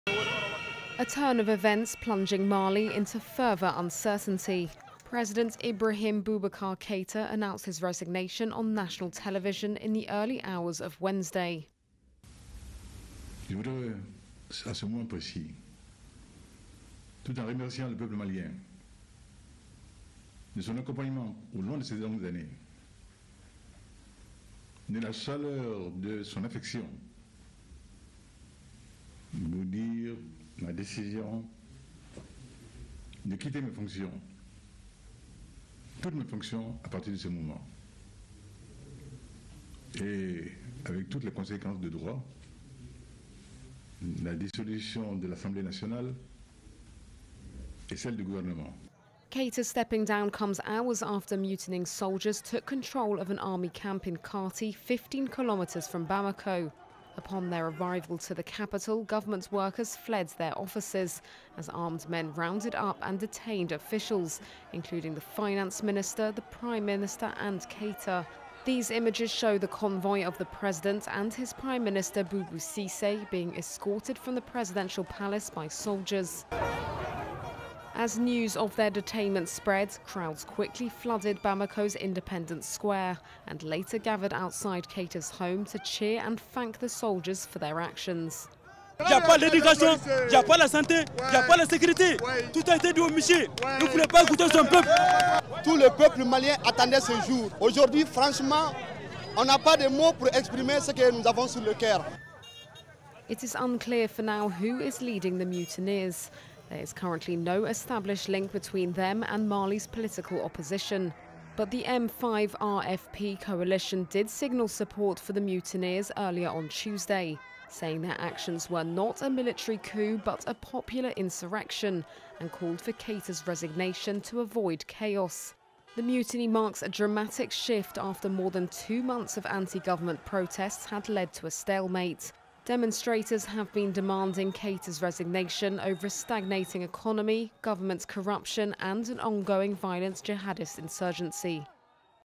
Mali's President Ibrahim Boubacar Keita resigns after military mutiny
Mali President Ibrahim Boubacar Keita resigned on Tuesday and dissolved parliament hours after mutinying soldiers detained him at gunpoint, plunging a country already facing a jihadist insurgency and mass protests deeper into crisis.